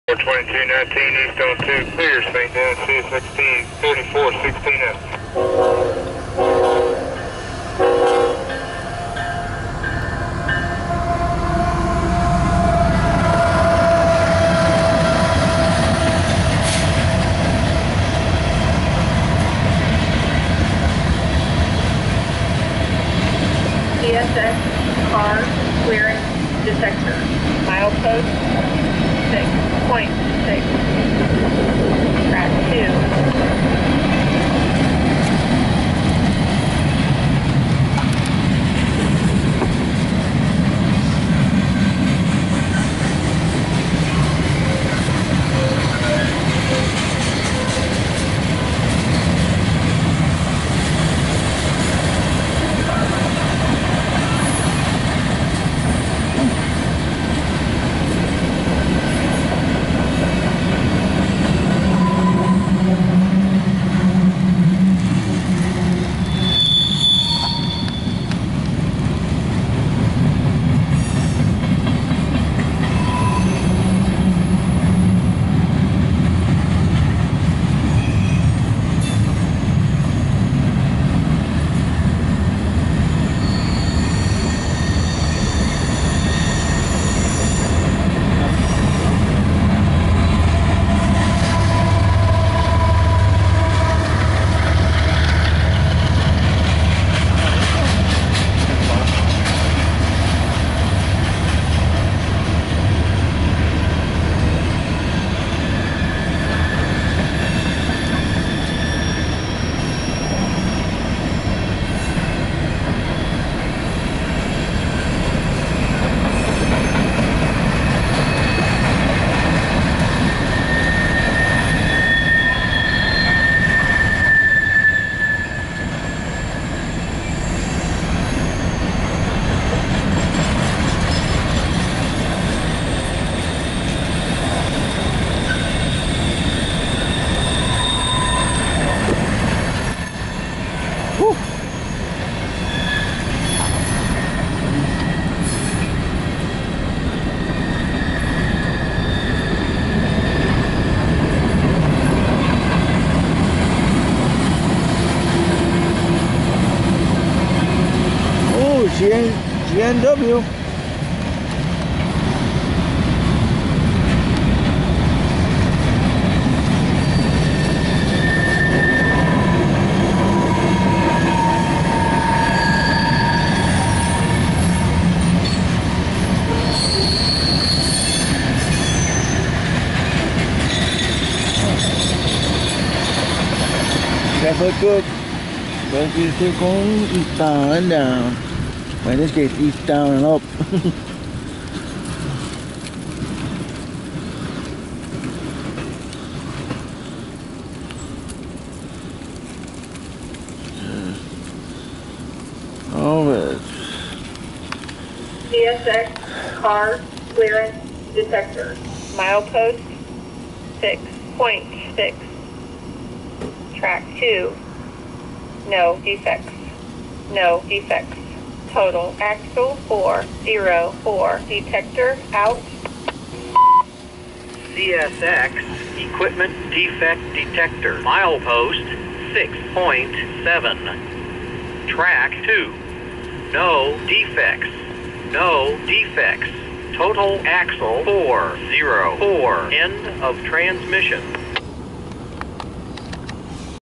#14720 CSX M422-19 Mix Freight train
CSX M422-19 Mix Freight train is heading eastbound on track 2